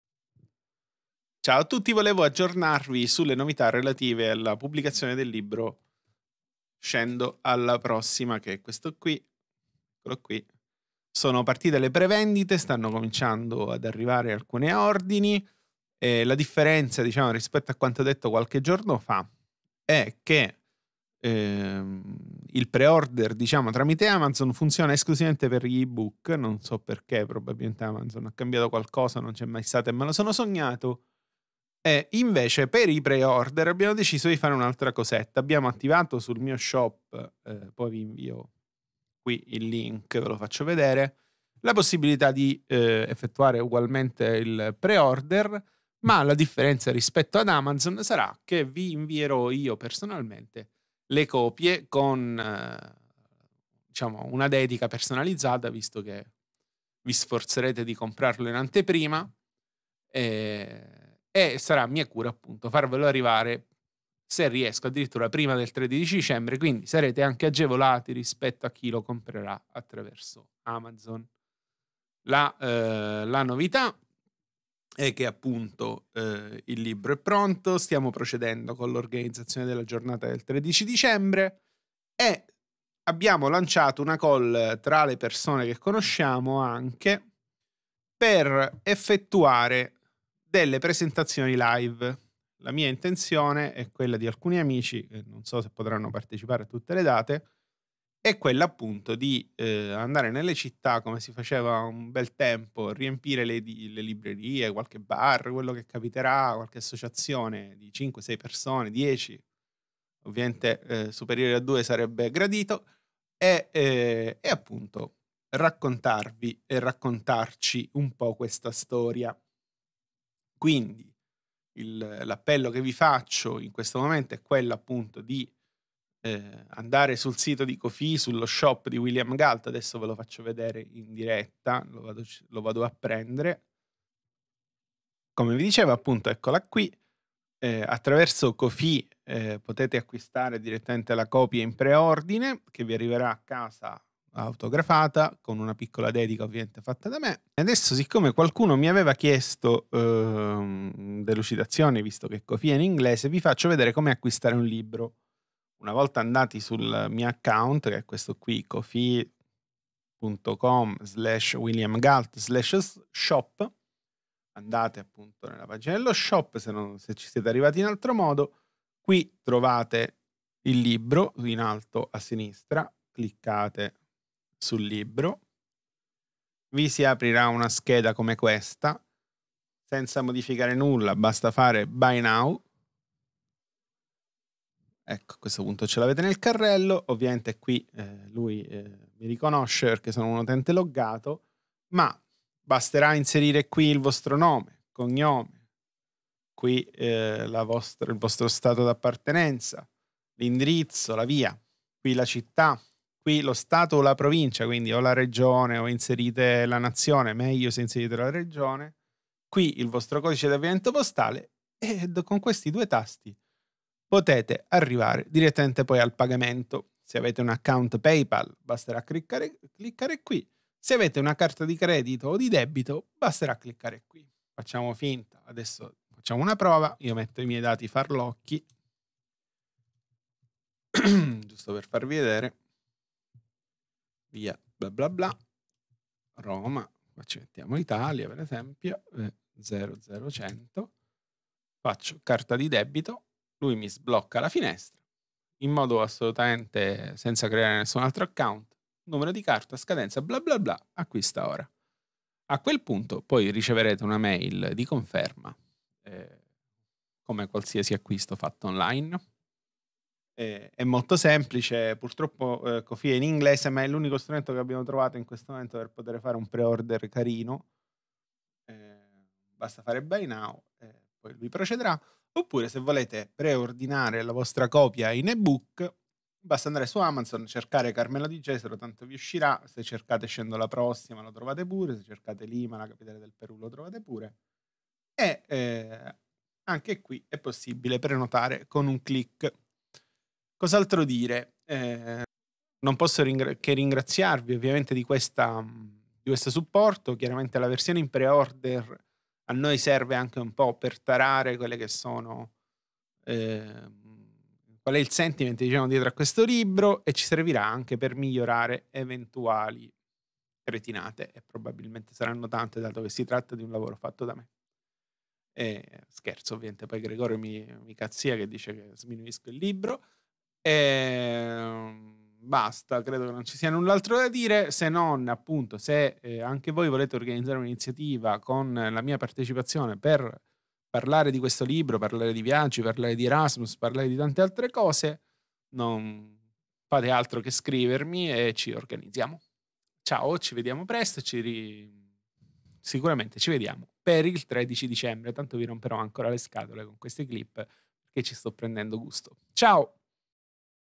Come ordinare una copia del libro
Se sei un boomer come me e tuttavia vuoi acquistare il mio libro, ecco un video che ti spiega come procedere al PRE-ORDER di "Scendo alla prossima. Lima: la capitale del Perù".

Questo è il link per prenotare l'ebook